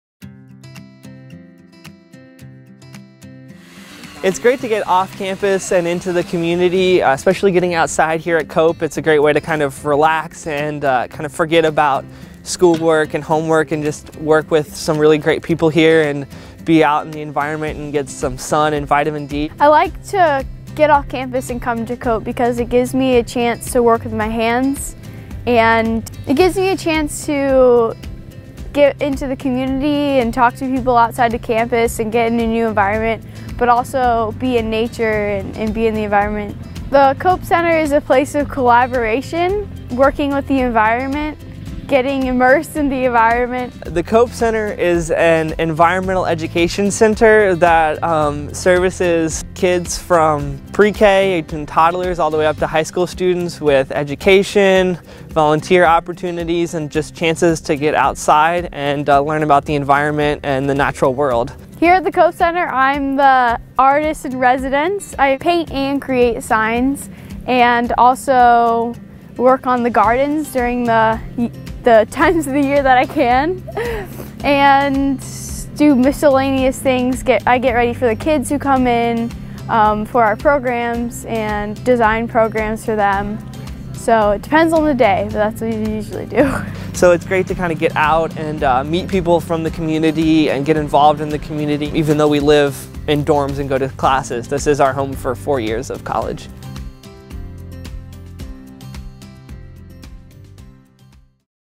Community Service at Earlham
Each year, Earlham’s students collectively contribute more than 23,000 reported hours (and many more unreported hours) of community service to support important local agencies and needs. The College is among 24 colleges hosting a Bonner Scholars Program that awards 15 first-year students with four-year scholarships for continued participation in community outreach; 60 Bonner Scholars are active during the current year.